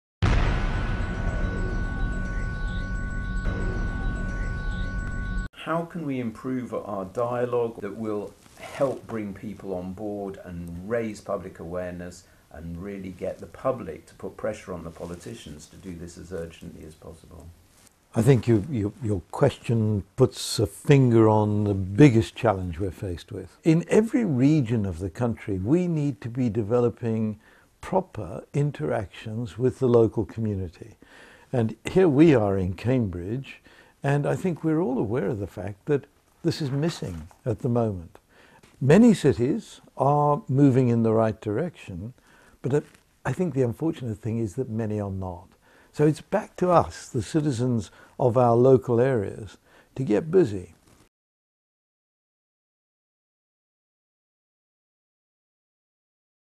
Prof. Sir David King on engaging communities to tackle climate change
Ahead of a panel discussion as part of the Cambridge Science Festival 2018, Terry Macalister asked Prof. Sir David King about how to engage more  people and politicians in tackling climate change